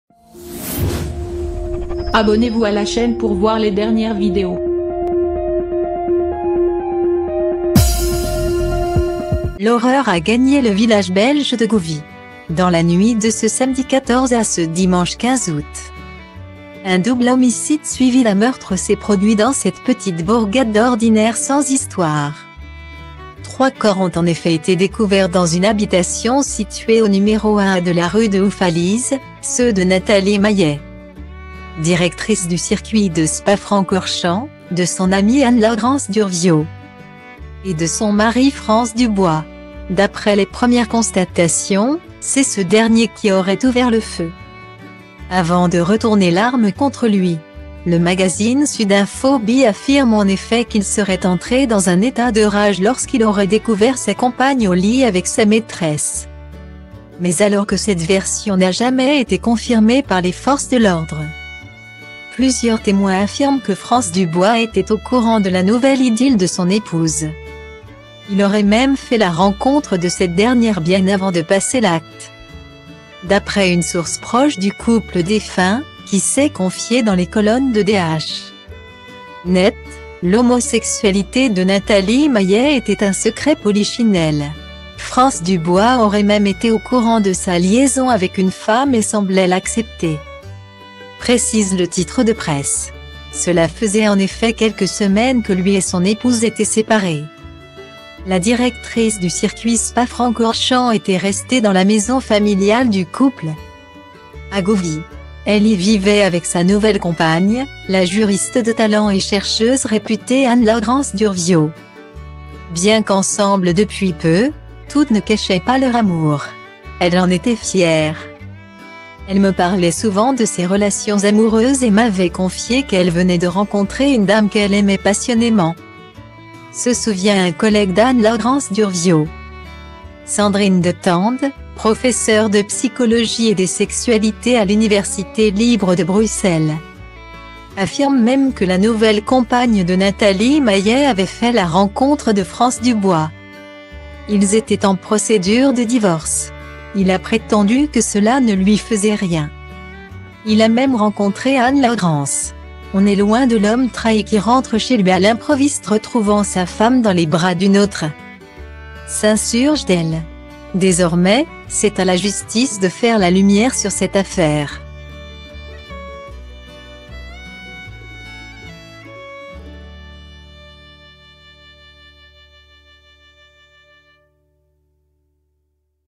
Nathalie Maillet tuée par son mari : ce que Franz Dubois dit au sujet de sa relation avec une femme
L'horreur a gagné le village belge de Gouvy. Dans la nuit de ce samedi 14 à ce dimanche 15 août,un double homicide suivi d'un meurtre s'est produit dans cette petite bourgade d'ordinaire sans histoire.Trois corps ont en effet été découverts "dans une habitation située au numéro 1A de la rue de Houffalize" : ceux de Nathalie Maillet,